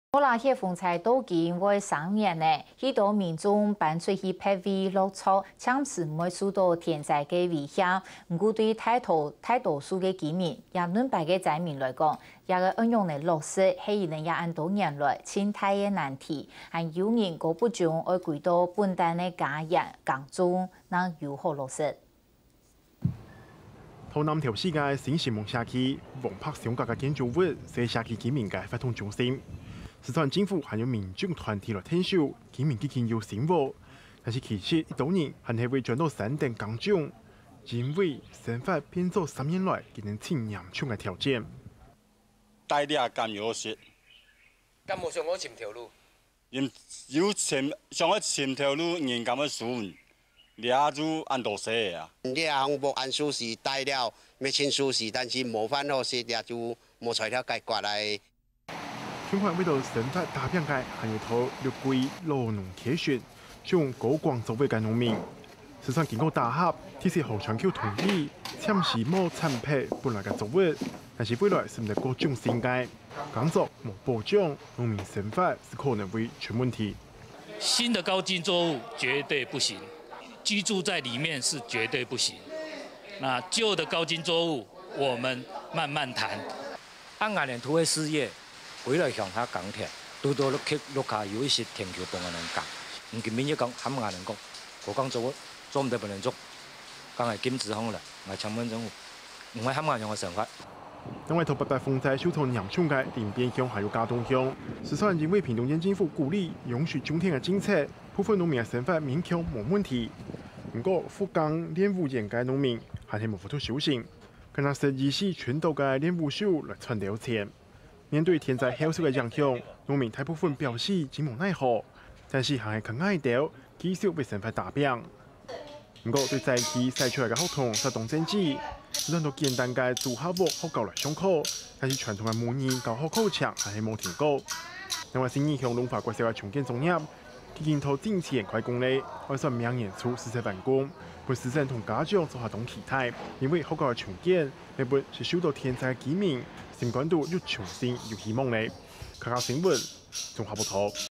2012「八八風災」系列追蹤報導：0503_88屆滿3週年 災民生計仍未完全復甦
莫拉克颱風所引發的八八大水，發生至今已經即將屆滿3週年，許多民眾搬離家園在別處安頓，暫時免於天災的威脅，但對於多數的居民而言，生計是這幾年面臨的最大難題，甚至有人選擇回到原本的家園務農只求溫飽。